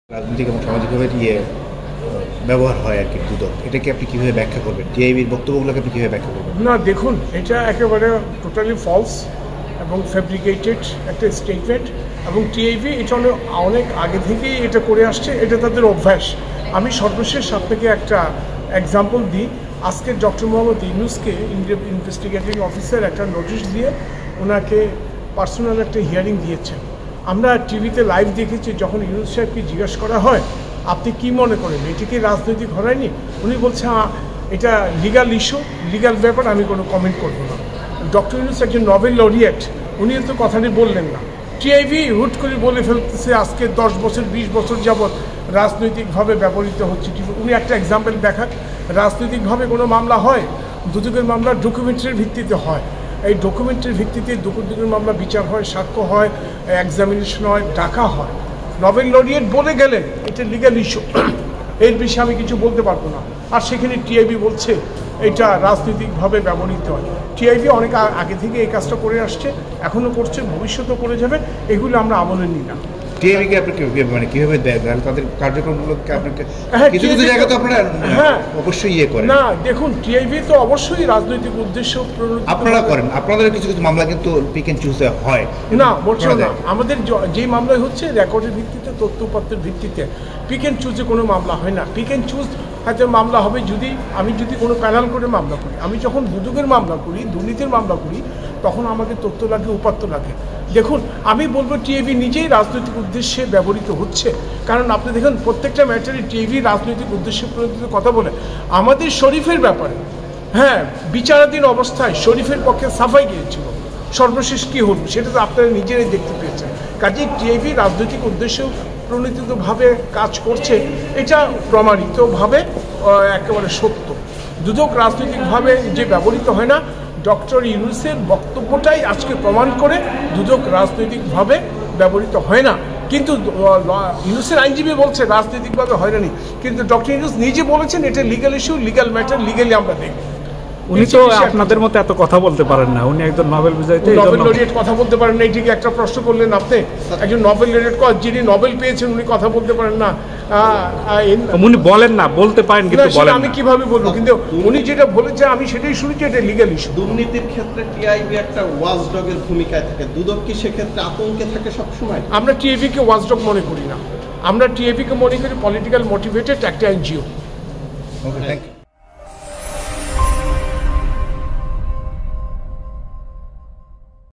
দুদক নয়, টিআইবি রাজনৈতিক উদ্দেশ্যে ব্যবহৃত হয় | Latest Bangla News | Dhaka Post
দুদক নয় বরং টিআইবি রাজনৈতিক উদ্দেশ্যে ব্যবহৃত হয়, ব্যবহৃত হচ্ছে বলে মন্তব্য করেছেন দুর্নীতি দমন কমিশনের প্রধান আইনজীবী খুরশীদ আলম খান .